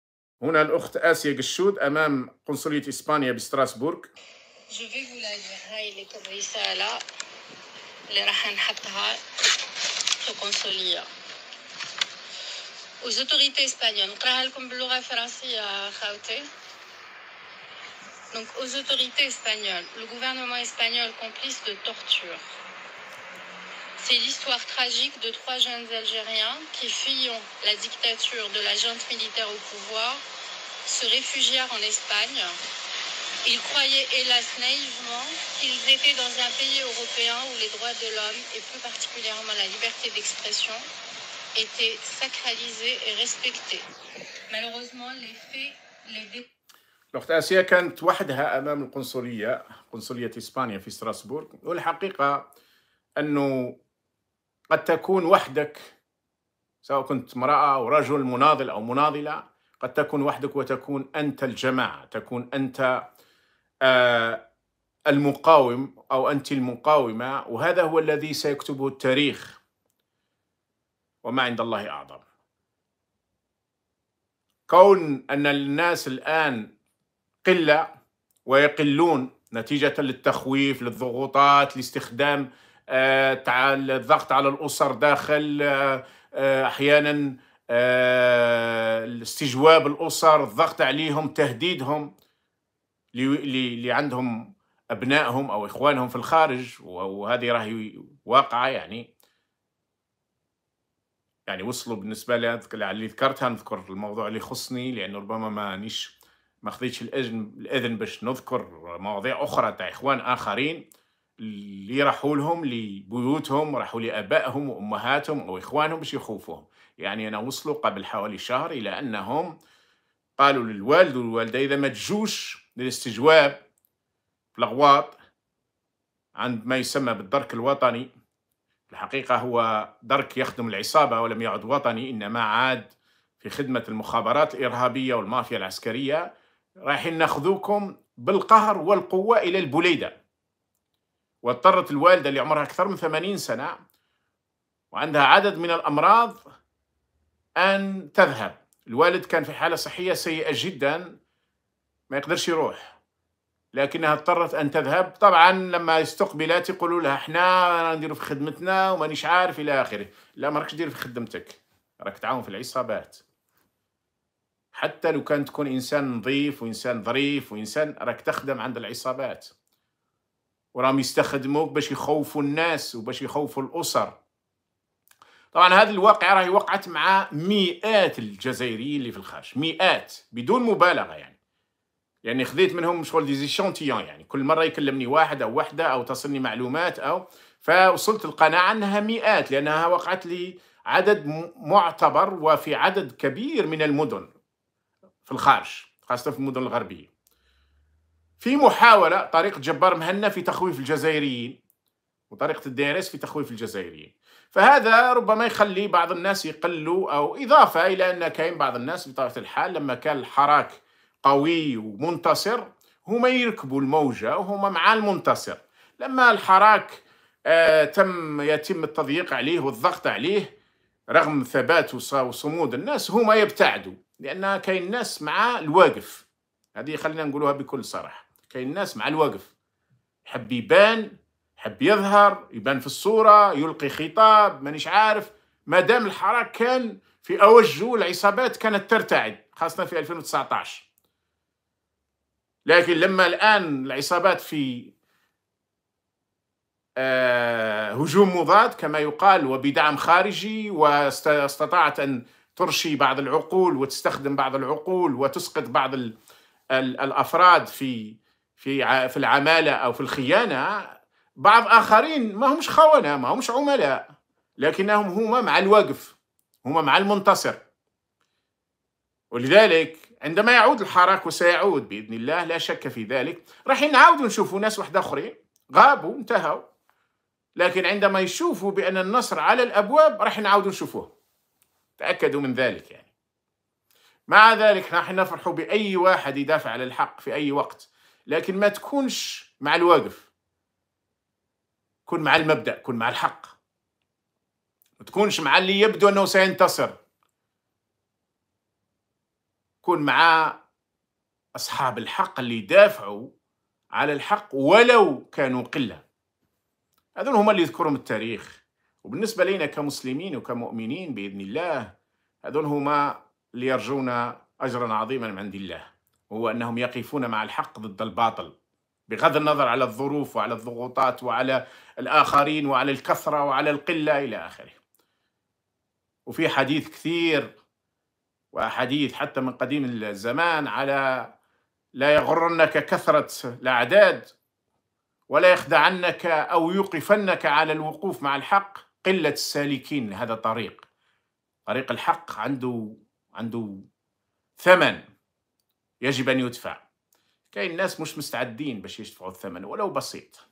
من أمام قنصلية إسبانيا بـ ستراسبورغ تنديداً بترحيل محمد عبدالله ومحمد بن حليمة وابراهيم لعلامي...
آسيا ڤشود من أمام قنصلية إسبانيا بـ ستراسبورغ للتنديد بترحيل محمد عبدالله ومحمد بن حليمة وابراهيم لعلامي...

 محمد العربي زيتوت 20 نوفمبر 2022